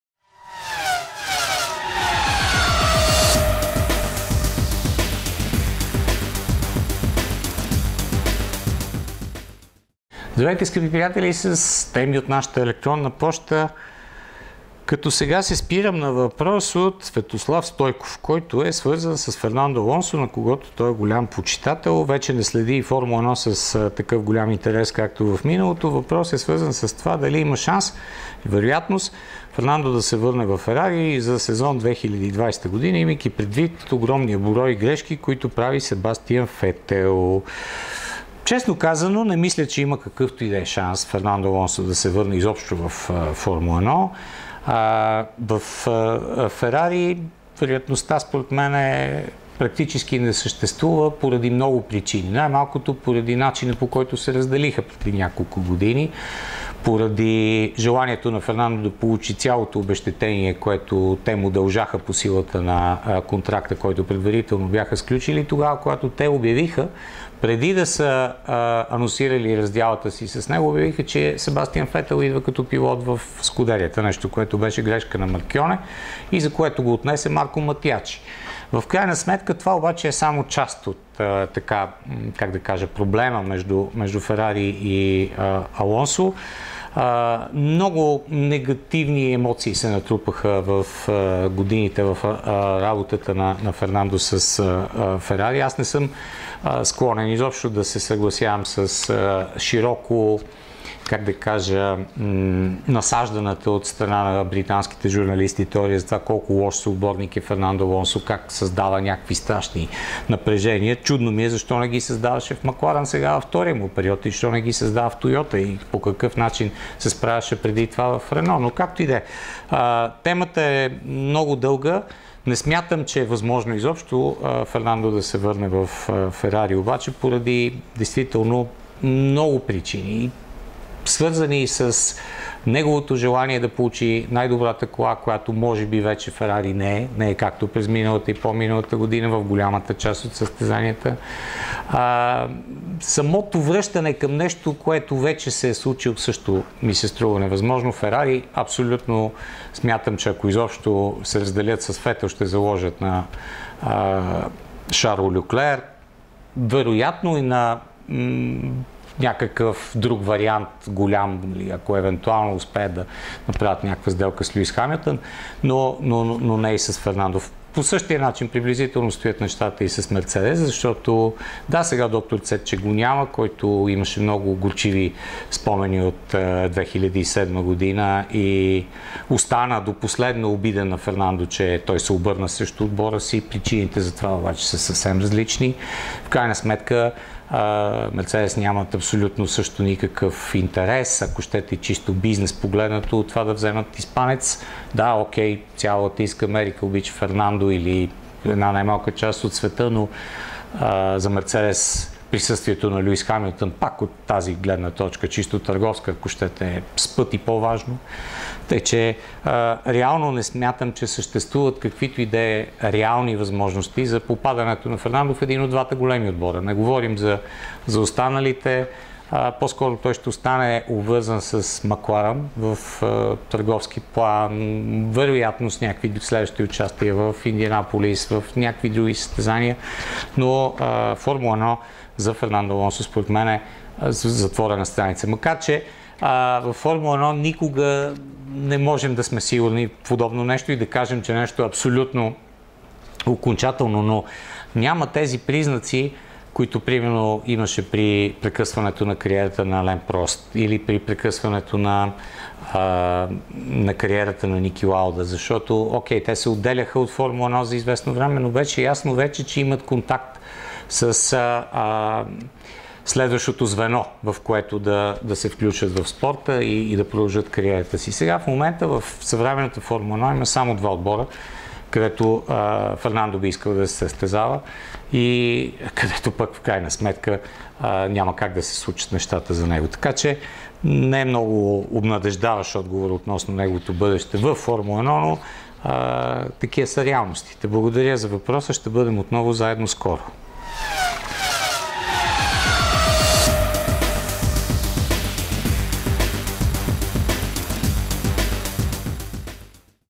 Има ли вероятност Алонсо да се завърне във Ферари?
Има ли някаква вероятност Фернандо Алонсо да се завърне във Ферари за сезон 2020? Имайки в предвид постоянните грешки които Фетел прави, не се ли изчерпва търпението на Ферари?